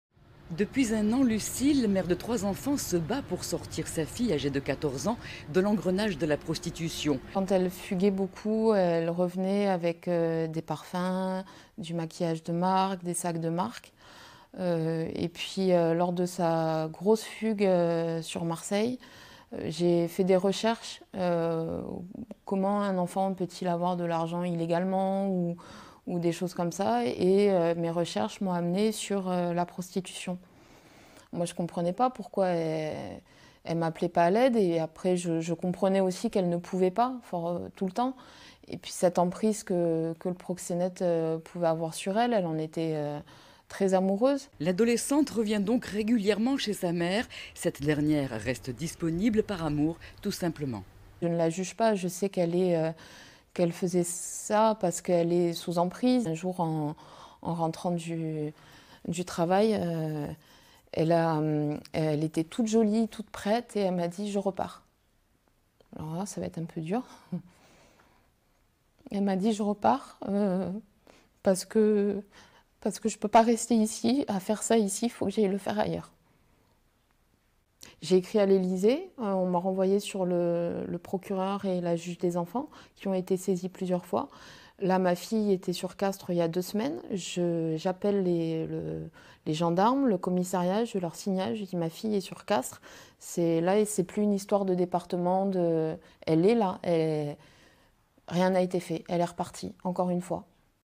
Témoignage : son combat pour sauver sa fille de la prostitution
Sa fille de 14 ans fugue régulièrement et se prostitue. Désespérée, une mère de famille a accepté de témoigner.

▶️ Article à lire sur France 3 Occitanie 📲

Crédit images : June Raclet